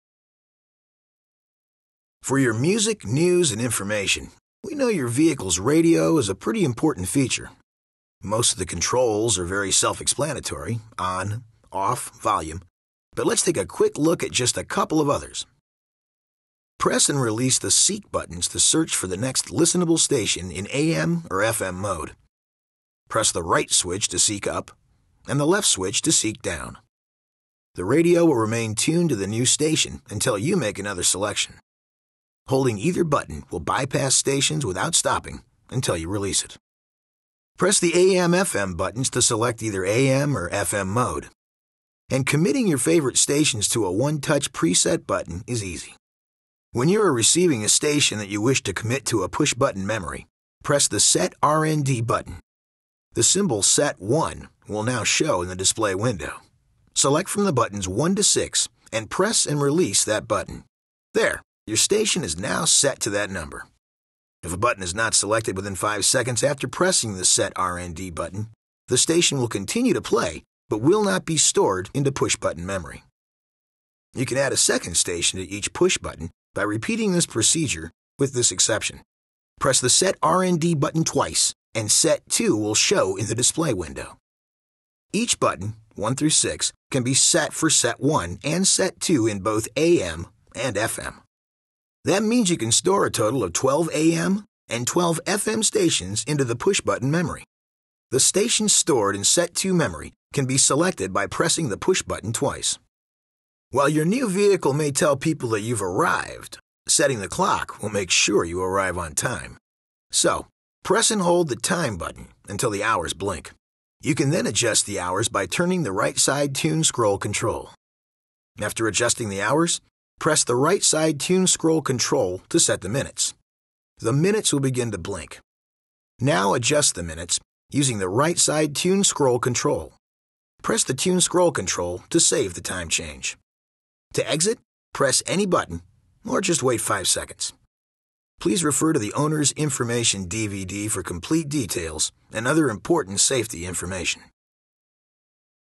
2011 Ram 1500/2500/3500 | Media Center 130 (RES)
This video describes the Media Center 130 (RES) in the 2011 Ram 1500/2500/3500 Truck.